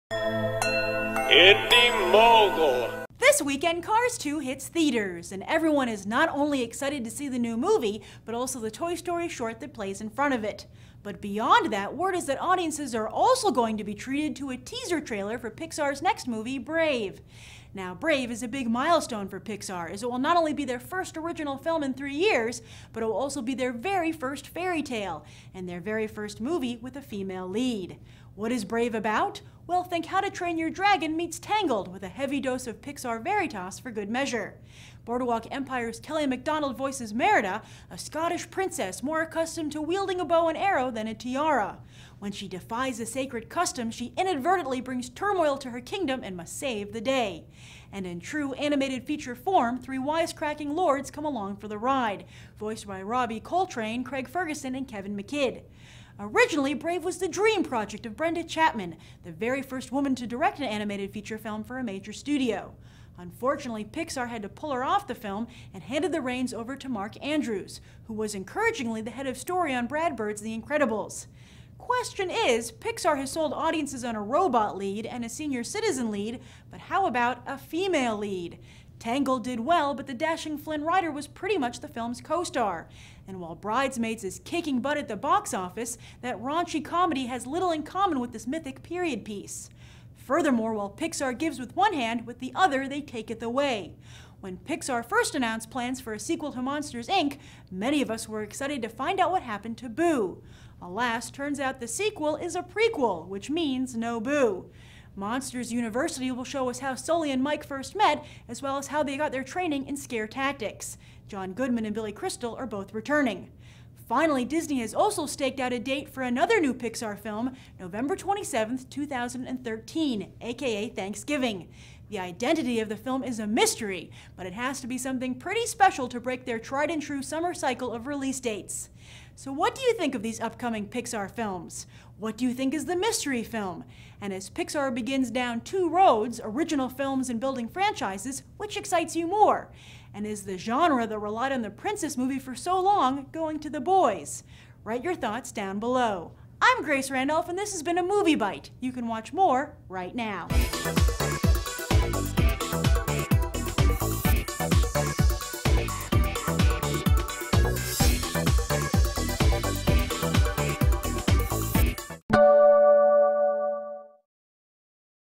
Pixar's Brave 2012, Monsters Inc 2 aka Monsters University: Beyond The Trailer
The teaser trailer for Disney Pixar's Brave hits theaters this weekend, and Beyond The Trailer host Grace Randolph tells you what to expect!  Plus find out what Pixar has planned beyond Brave in 2012, such as Monsters Inc 2 aka Monsters University!  Is the future of Pixar original films like Brave, or sequels like Cars 2 and Monsters Inc 2 aka Monsters University?  Enjoy Brave 2012 and Monsters University 2013 from Disney Pixar!